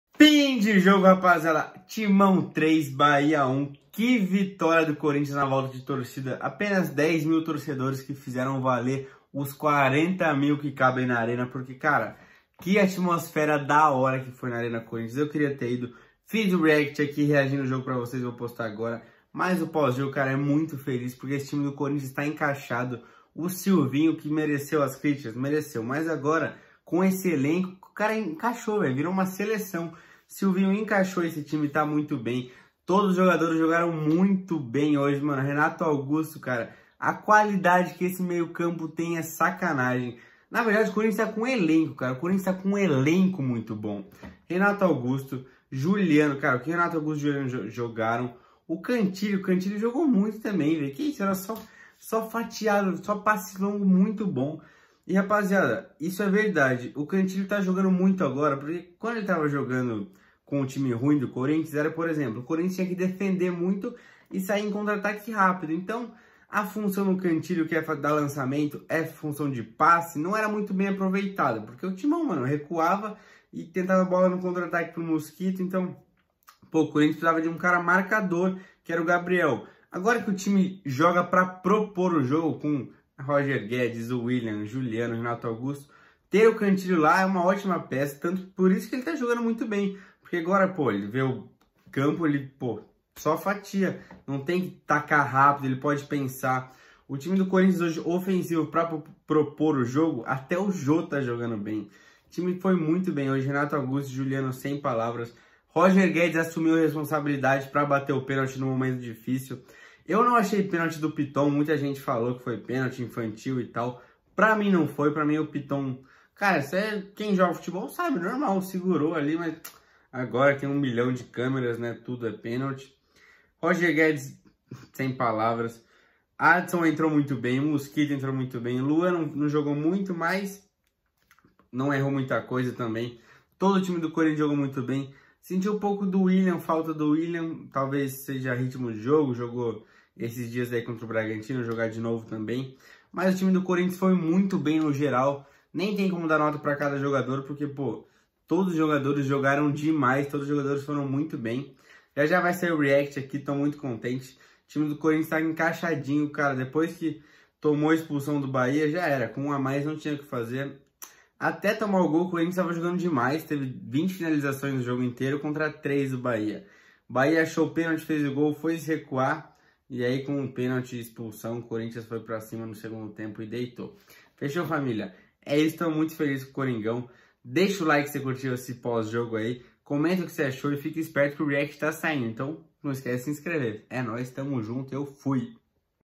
EU TÔ SYLVINIZADO!! Análise pós jogo Corinthians 3 x 1 Bahia
Neste vídeo falo minha opinião sobre os melhores momentos da partida entre corinthians e bahia pelo campeonato brasileiro com volta do público